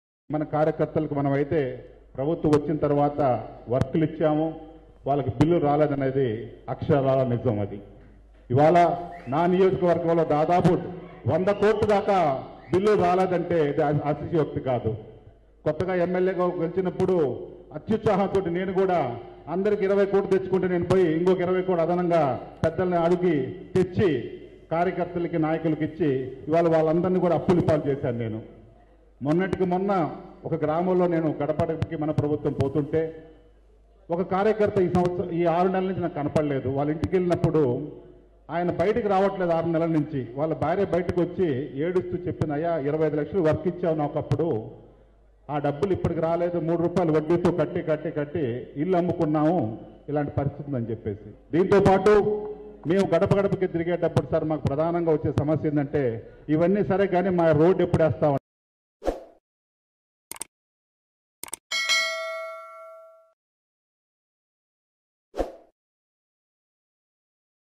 జగన్ పాలన చెత్తగా ఉందంటూ దర్శి వైసీపీ ఎమ్మెల్యే సంచలన వ్యాఖ్యలు..|| AP23 NEWS
జగన్ పాలన చెత్తగా ఉందంటూ దర్శి వైసీపీ ఎమ్మెల్యే సంచలన వ్యాఖ్యలు